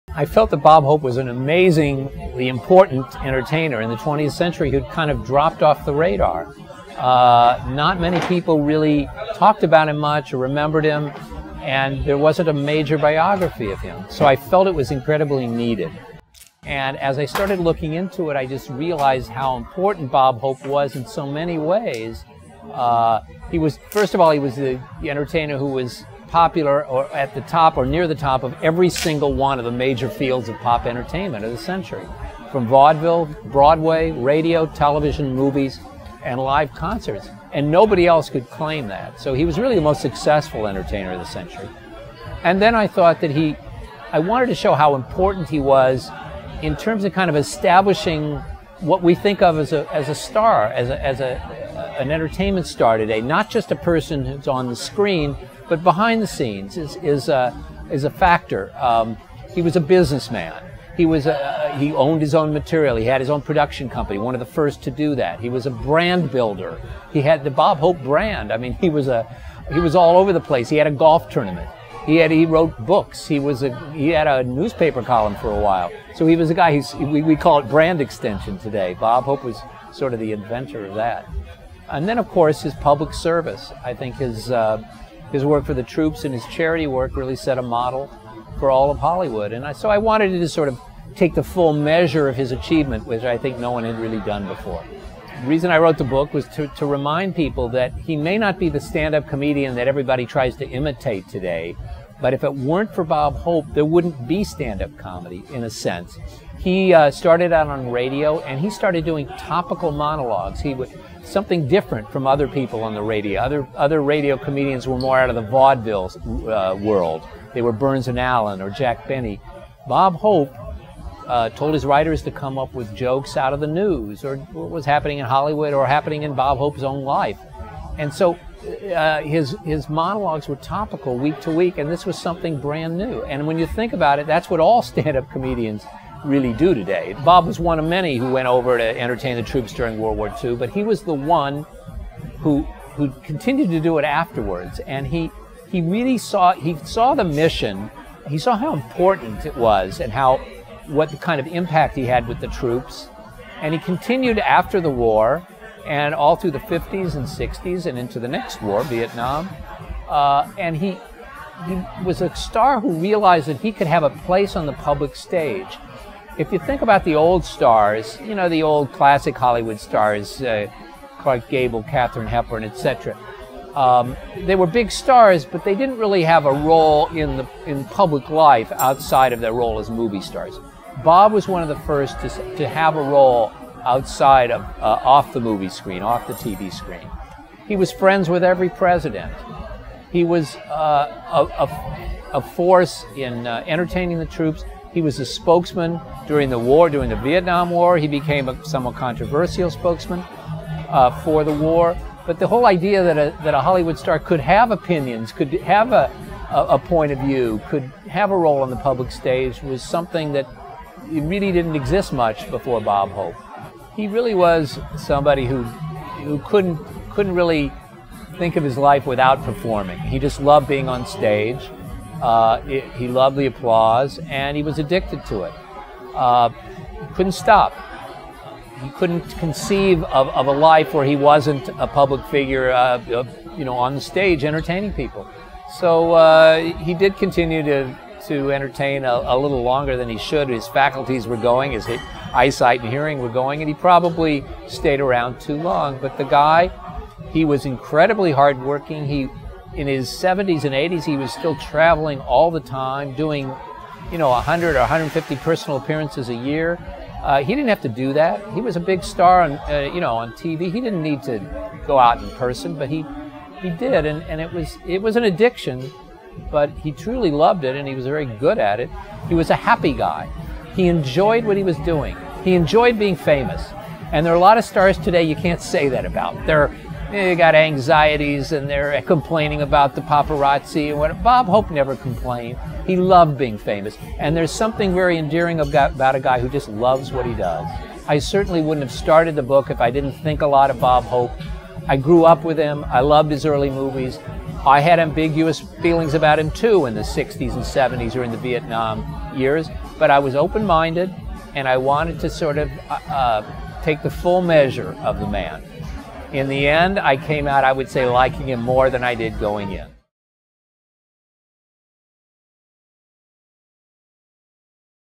Bob Hope: Entertainer of the Century
Go behind the scenes with Richard Zoglin, author of Bob Hope: Entertainer of the Century, a biography of the legendary comic and entertainer. The book is both poignant and revealing of Hope's personal life and his style as a performer. Hope is one of the first in Hollywood to have both a public persona on-screen and off. The author speaks in his own words about the man, and writing the book.

Filmed and Edited by Brendan Kownacki
for Hollywood on the Potomac